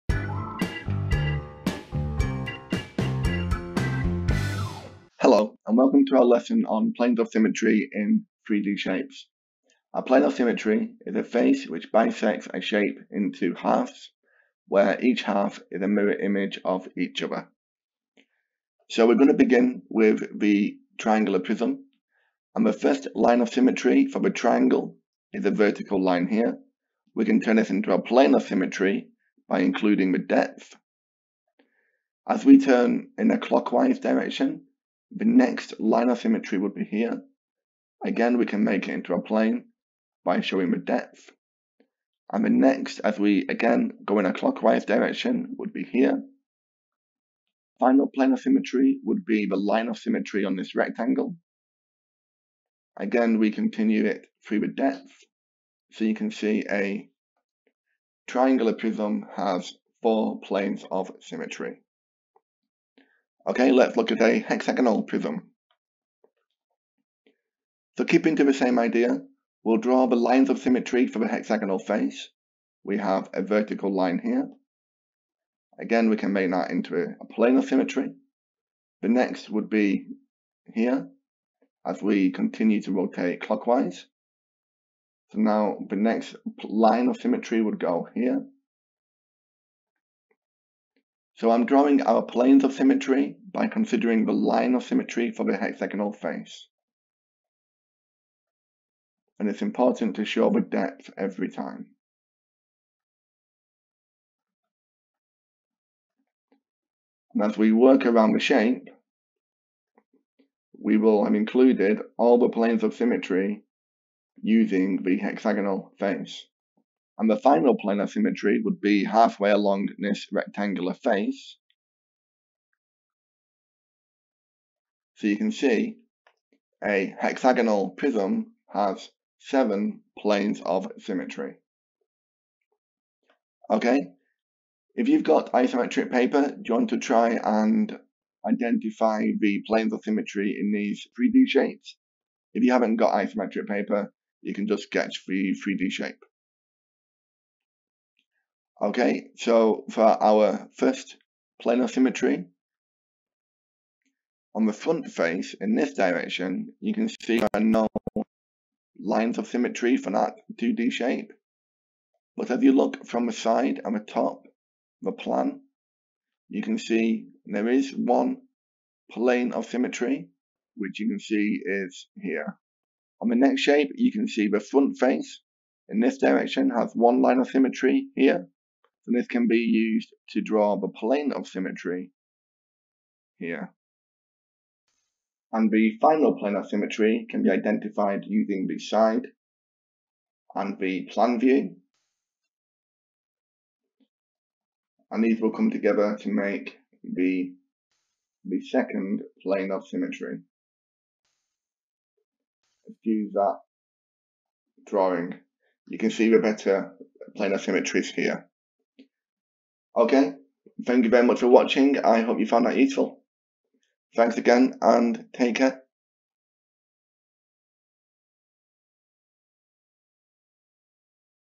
Planes of Symmetry in 3D Shapes | GCSE Maths | Mr Mathematics
How to identify the planes of symmetry in 3D shapes

The full lesson includes a starter, 2 mains, plenary and a worksheet.  Members of Mr Mathematics can download this